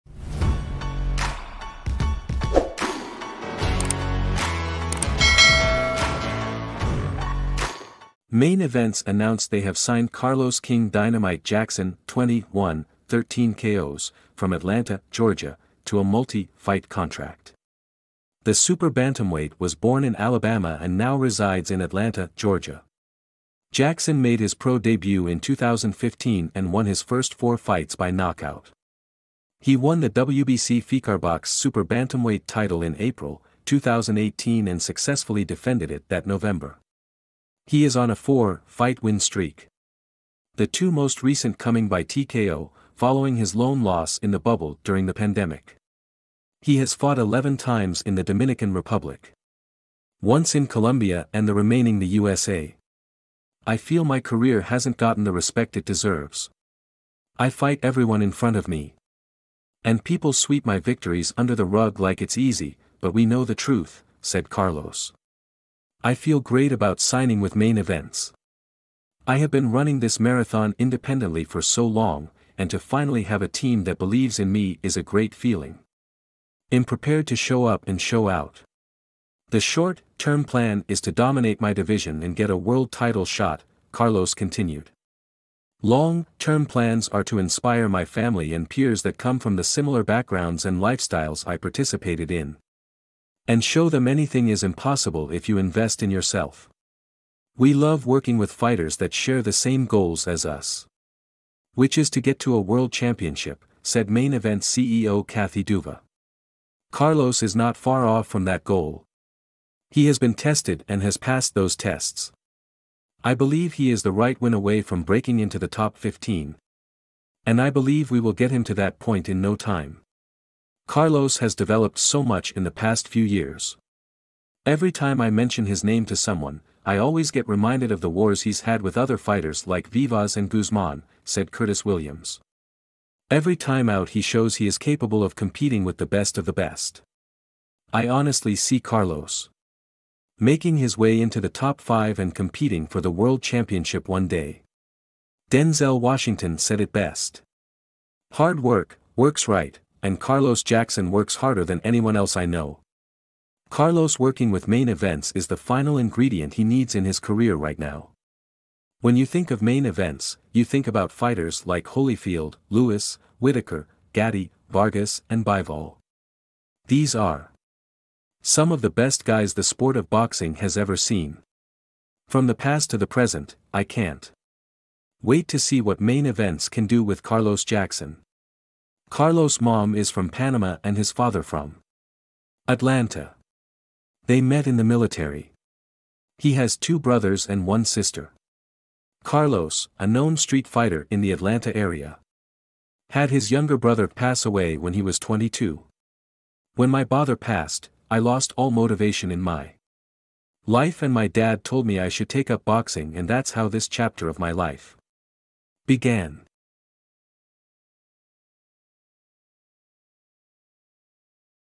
Main Events announce new super bantamweight signing - Boxing News
Main Events announced they have signed Carlos “King Dynamite” Jackson (20-1, 13 KOs) from Atlanta, Georgia, to a multi-fight contract. The super bantamweight was born in Alabama and now resides in Atlanta, Georgia. Jackson made his pro debut in 2015 and won his first four fights by knockout. He won the WBC FECARBOX Super Bantamweight title in Ap